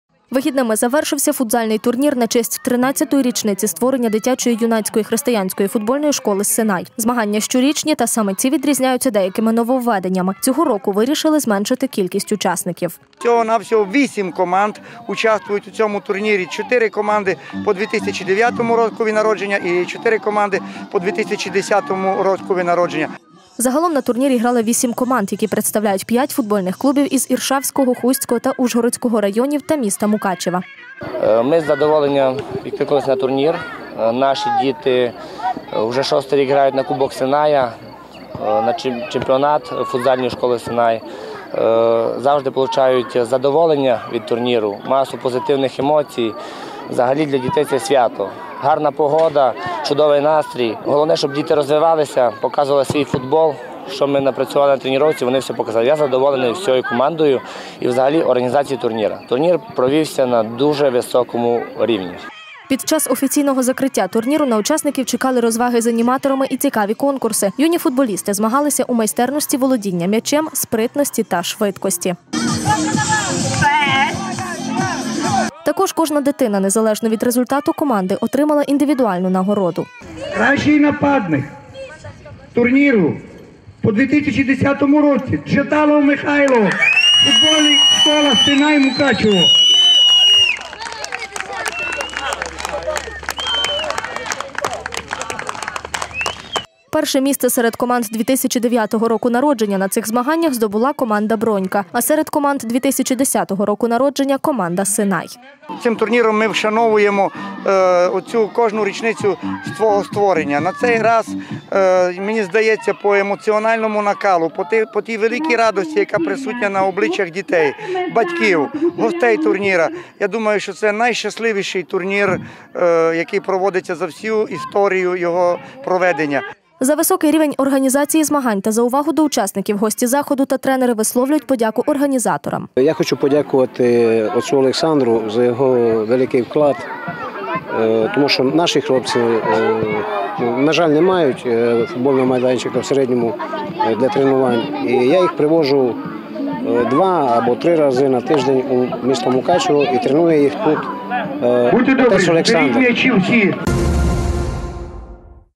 Закриття турніру
Тринадцятий футзальний турнір на честь річниці створення дитячої юнацької християнської футбольної школи «Синай» завершився цими вихідними. На офіційному закритті для дітей влаштували справжнє свято із танцями, конкурсами та призами.